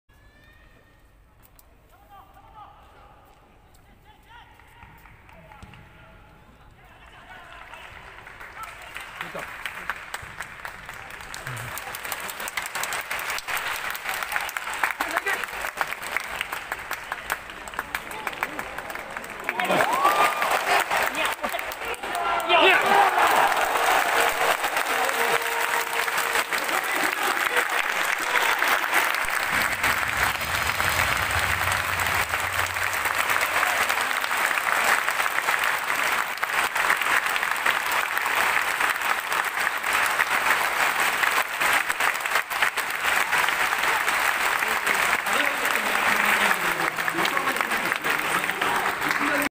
20200722横浜F・マリノス vs 横浜FCエジガルゴール！
2020Jリーグディビジョン1 第6節横浜F・マリノス vs 横浜FC 4-0 @日産スタジアム

エジガルゴール！